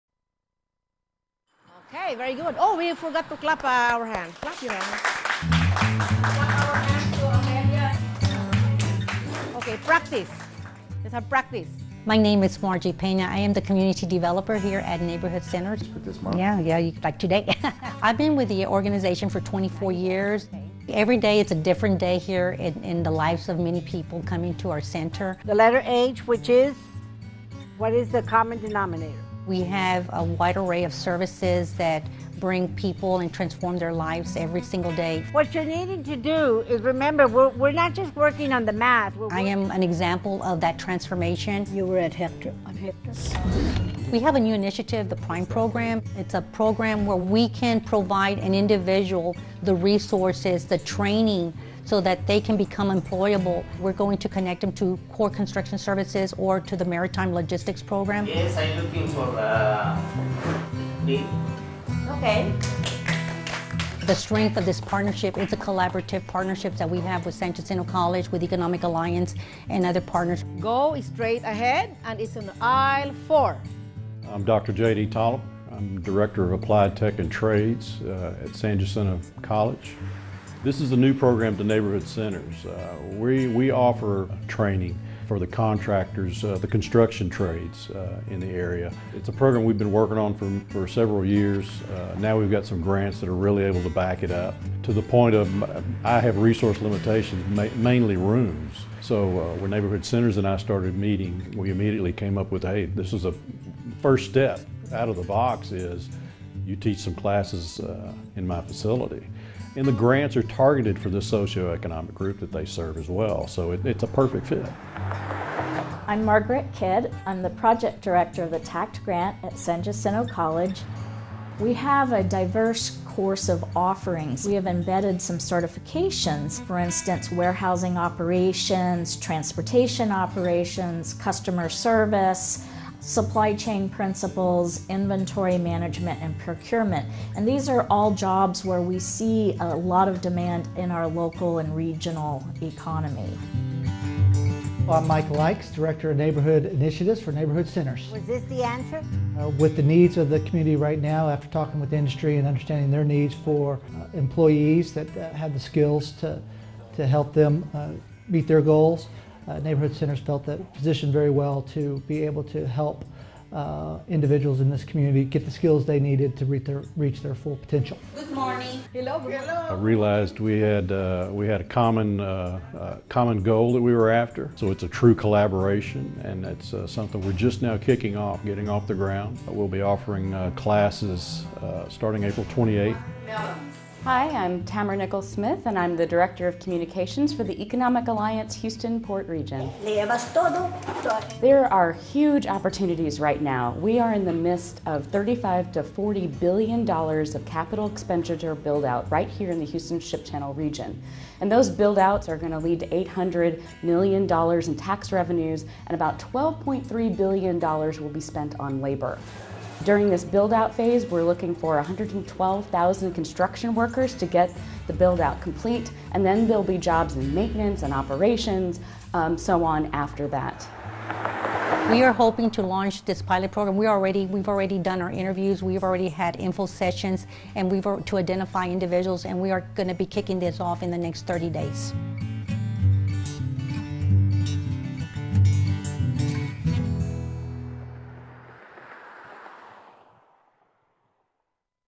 Neighborhood Centers Job Skills Training Partnership
The Cleveland-Ripley Neighborhood Center is partnering with San Jacinto College to provide on-site training classes at Cleveland-Ripley in the Maritime & Logistics and Construction fields. Come find out more about the careers available in your area and how you can get the skills required by top employers.  For questions please call The center is hosting information sessions: Saturday, March 21st from 11:00am-12noon, and Tuesday, March 24th from 6:30pm-7:30pm